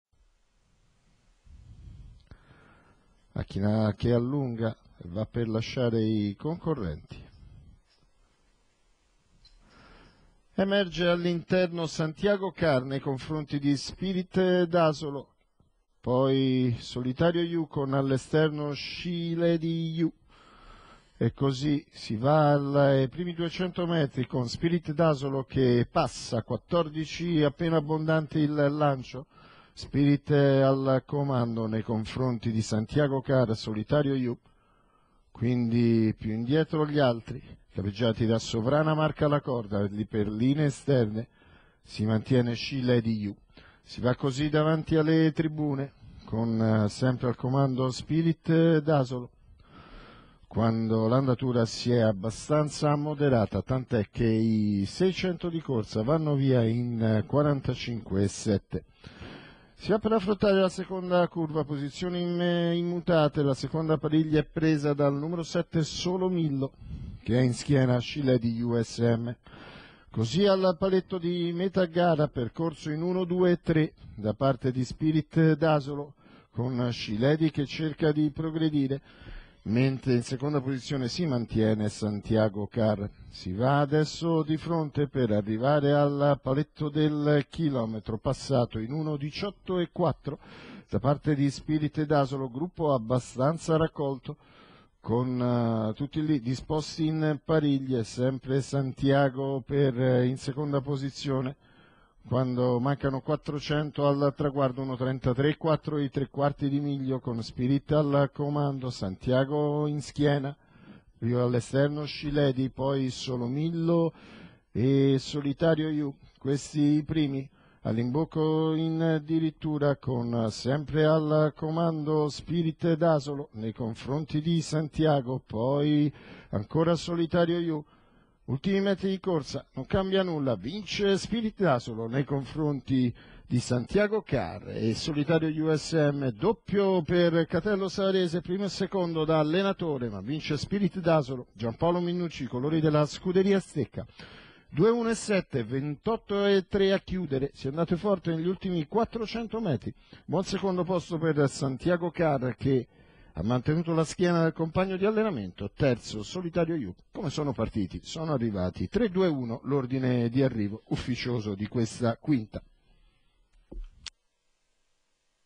31/05/2014, Roma , Premio Mint Di Jesolo C.naz
1. SPIRIT D'ASOLO (MINNUCCI GP.), 2. SANTIAGO CAR (PETTINARI F.), 3. SOLITARIO YOU SM (BECCHETTI LUCIO), 4. SCI LADY YOU SM (BATTISTINI D.), 5. SIRO DI SGREI (D'ALESSANDRO V. JR), 6. SOVRANA MARK (CUGLINI D.), 7. SANSONE SCO (PIETROPAOLI P.), 8. SUPER SIC (MUSCOLINI ROBERTO), 9. SUPER OK (MASCIULLO A.), 99. SOLOMILLO (LO VERDE G.)